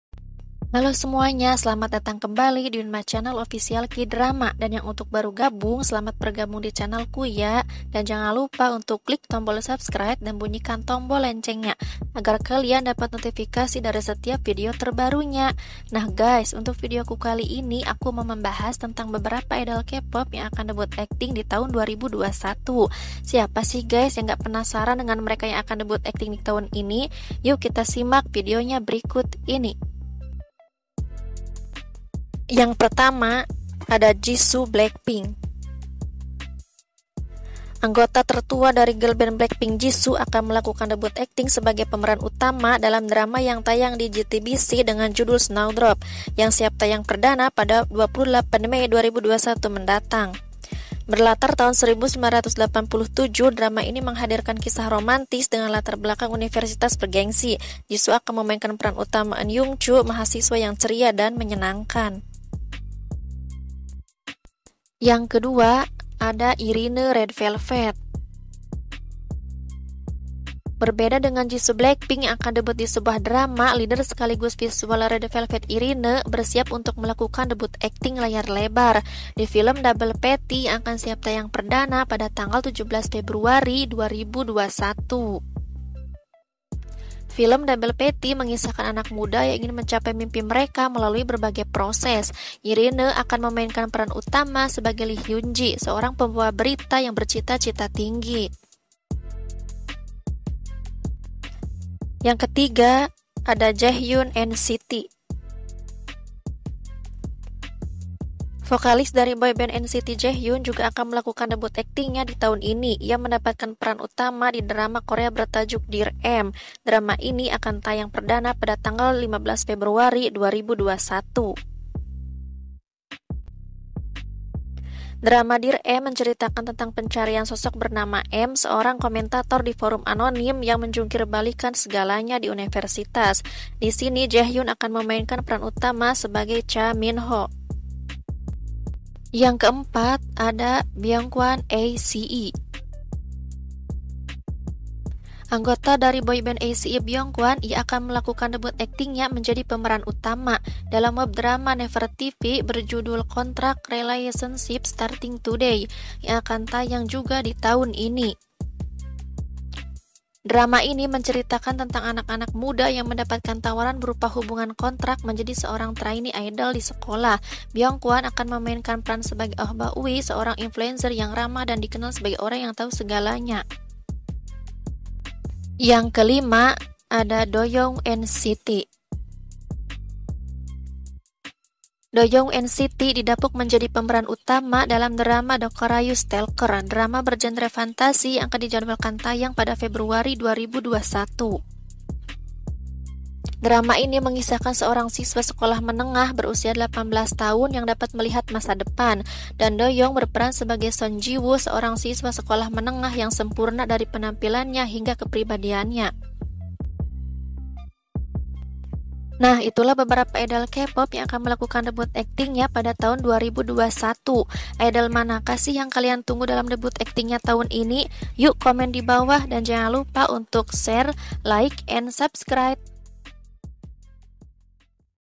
5 Idol K-Pop Yang Akan Debut Akting Di Tahun 2021
hallo semuanya selamat datang kembali di Yun my channel...

nah untuk kali ini aku mau bahas tentang idol K-Pop yang akan debut akting tahun 2021.

mau tau siapa ajah yu simak videonya!